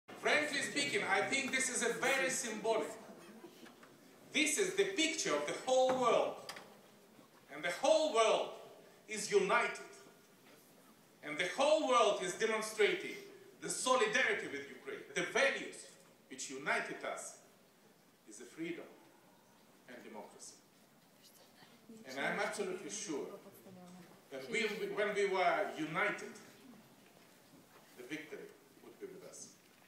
Porky in Zurich on… The Victory.
Ukro Preznik Poroshenko Speech in Zurich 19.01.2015